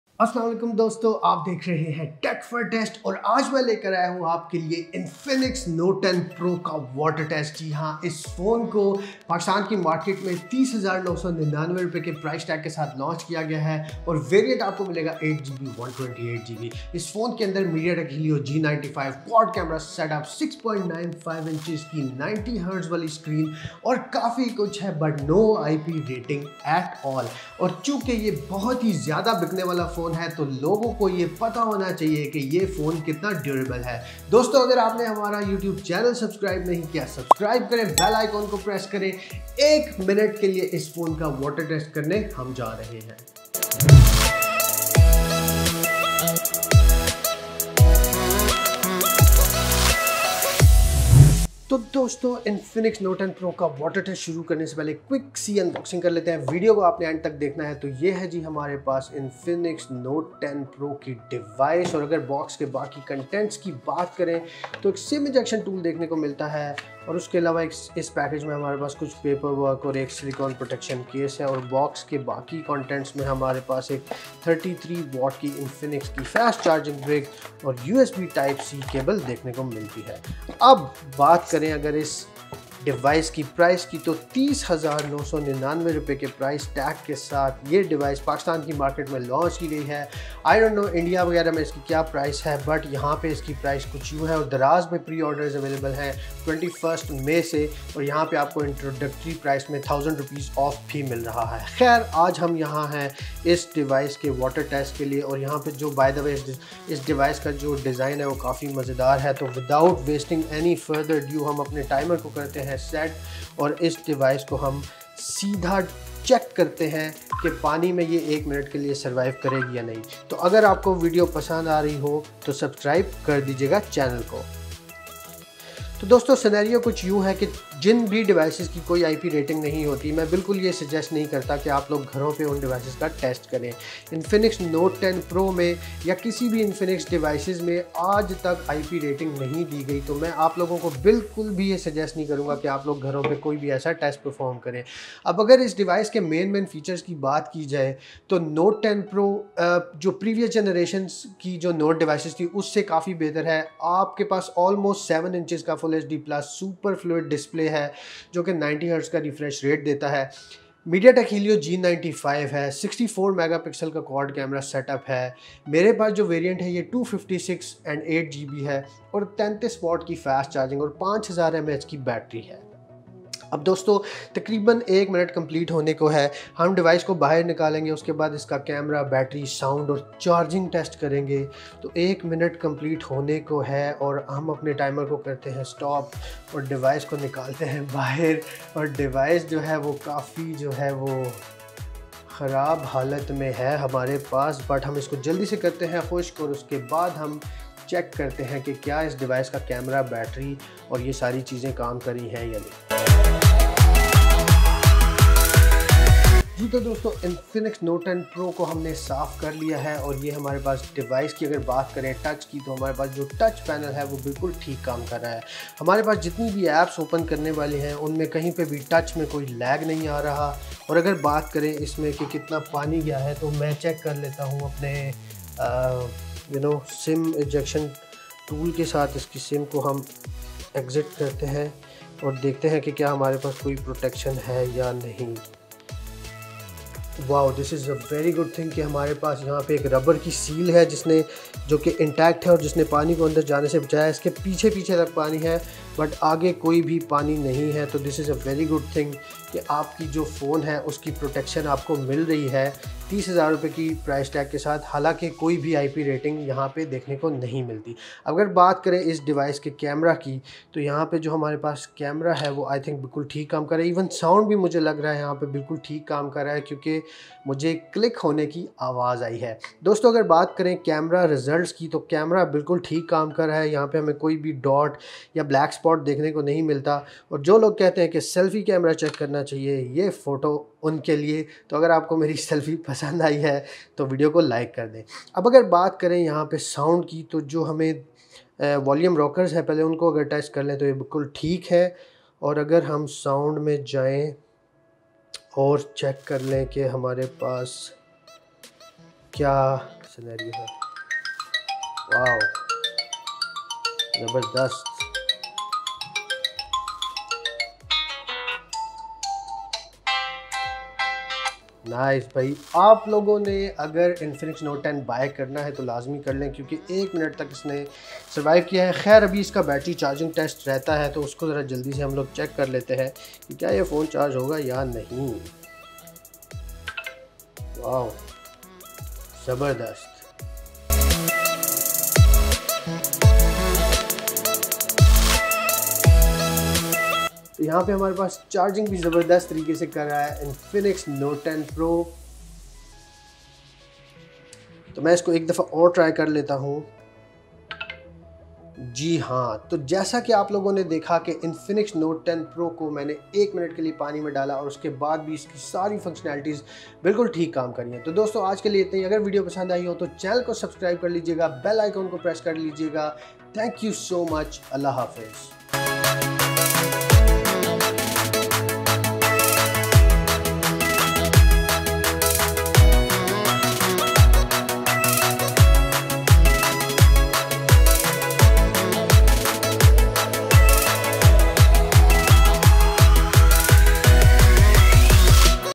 Infinix Note 10 Pro Water Test & Unboxing | Infinix Note 10 Pro Durability Test
Infinix Note 10 Pro Water Test Video is here. Infinix Note 10 Durability Test has been performed into this video by putting it into water for 60 seconds.

This device has a price tag of 30,999 and you can pre-order this device from Daraz.pk with a discount of 1000 and a giveaway. This phone offers allot in terms of specifications but i wanted to see if Infinix Note 10 Pro is waterproof or not.